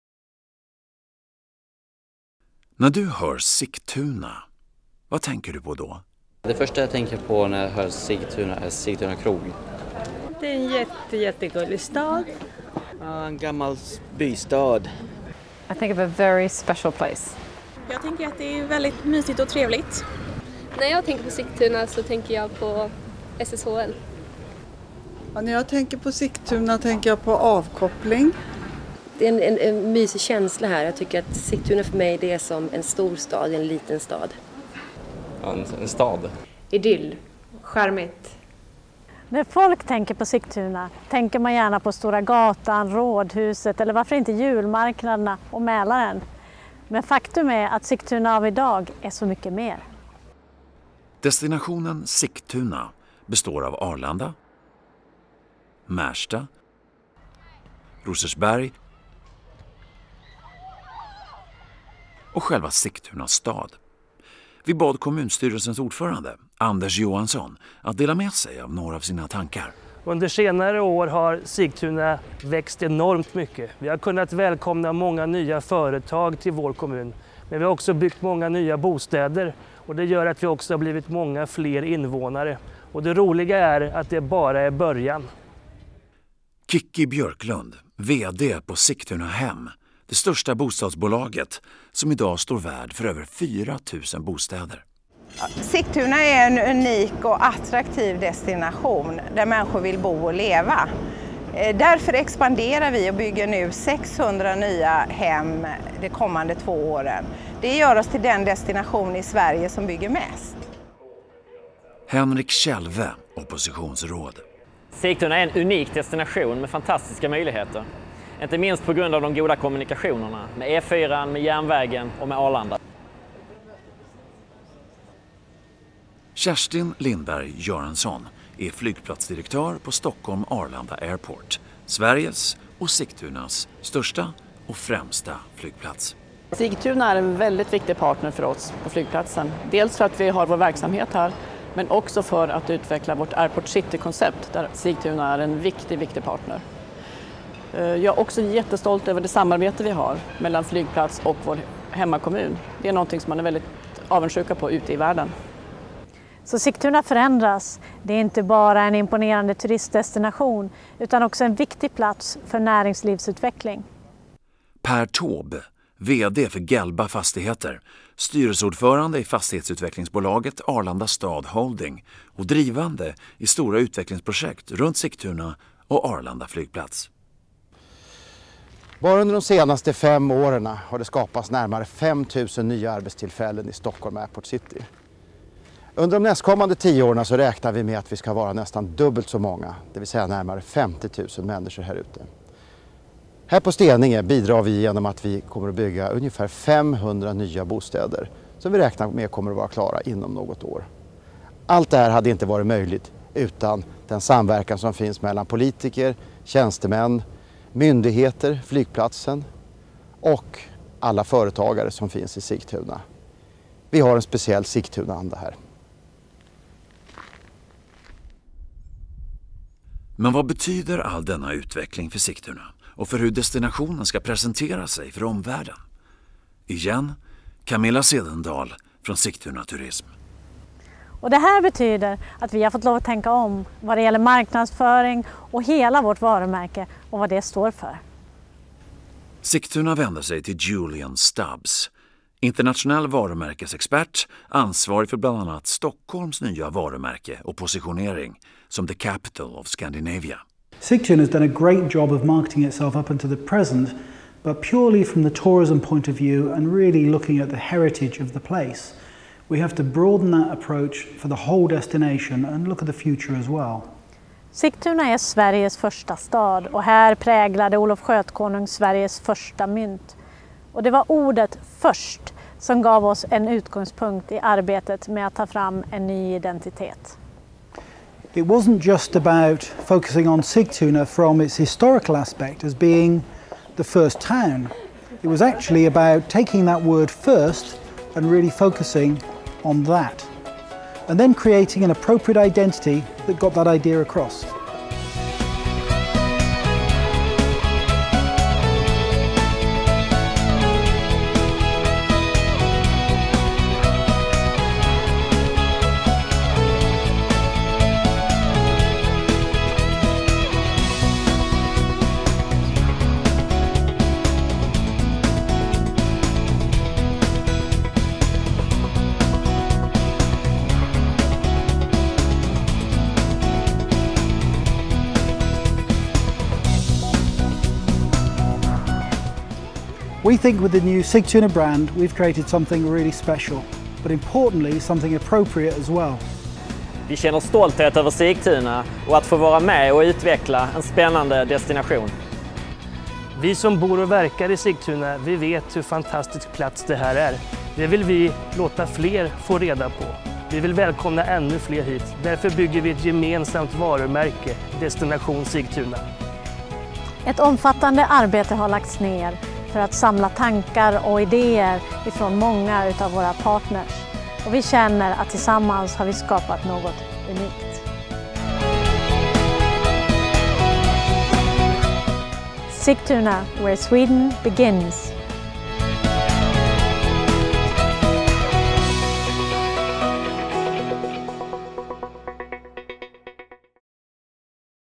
Destination Sigtuna nytt varumärke 2011
Den 1/1 2011 lanserade Destination Sigtuna ett nytt varumärke.